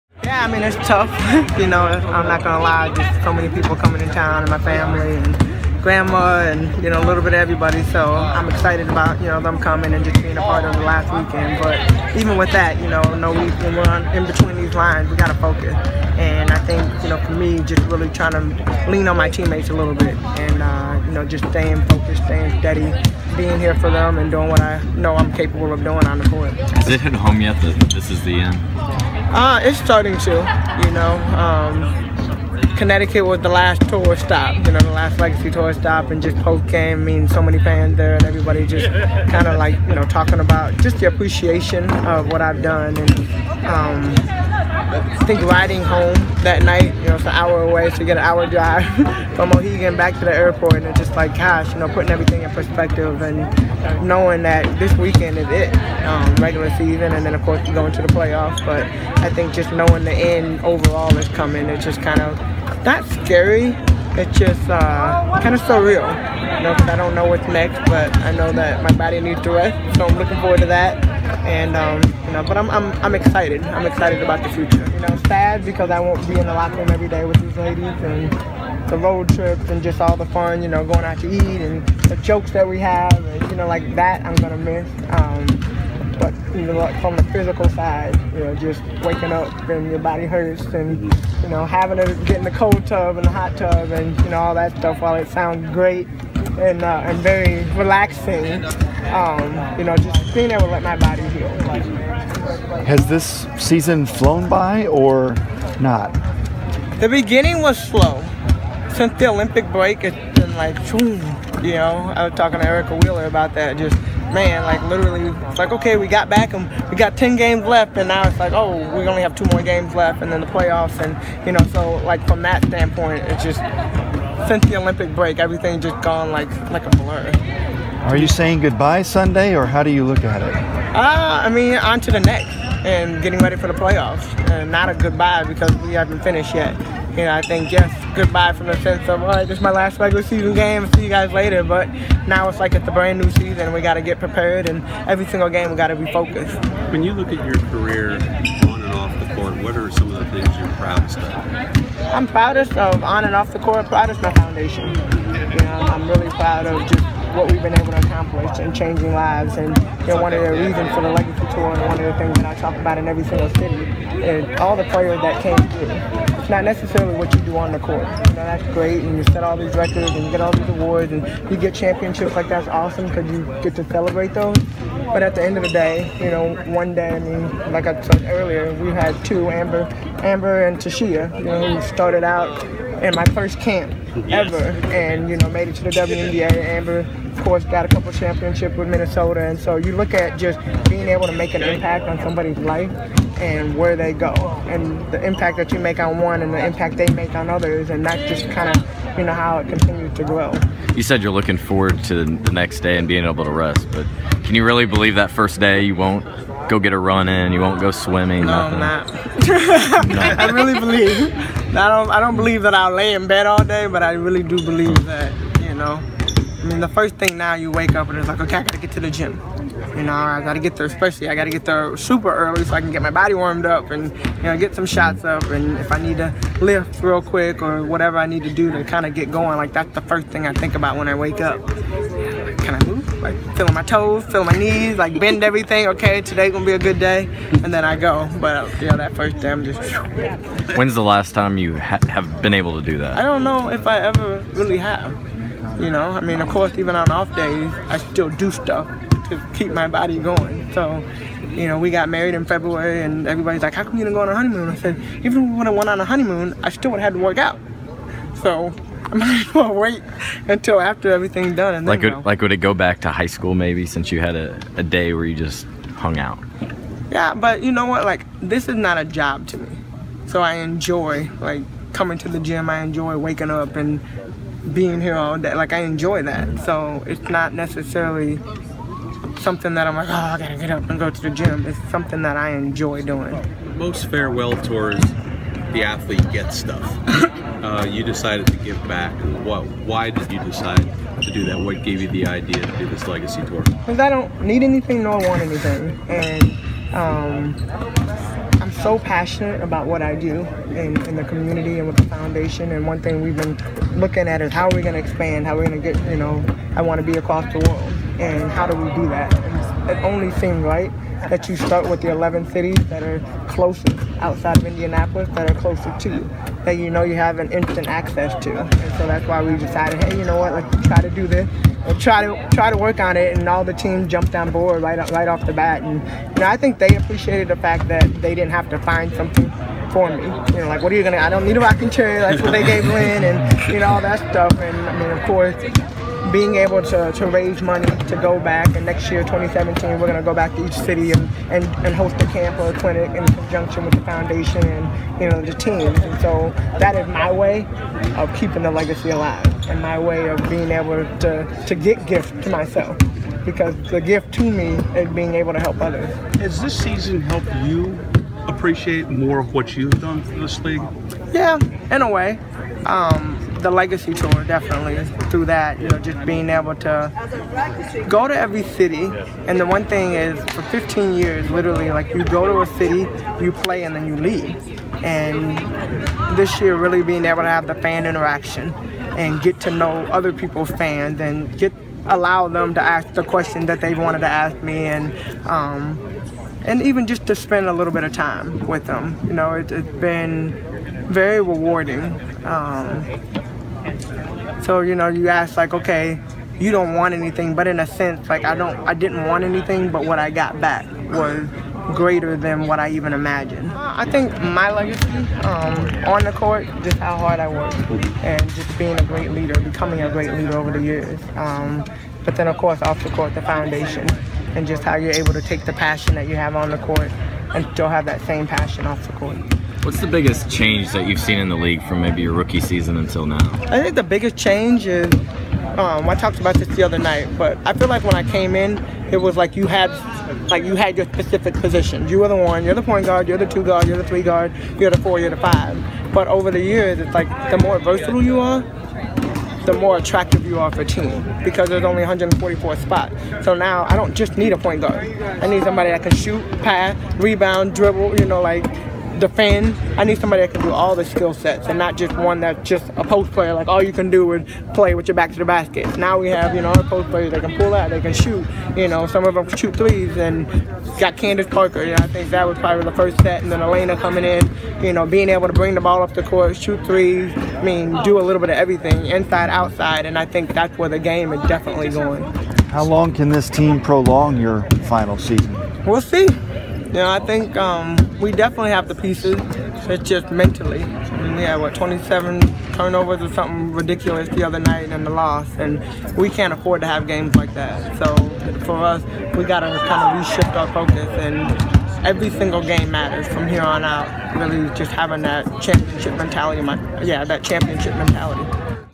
Tamika Catchings prepares for her final regular season game
Tamika Catchings has two regular season games left in her professional basketball career. Then, it's on to the playoffs. After that? Even Tamika doesn't know.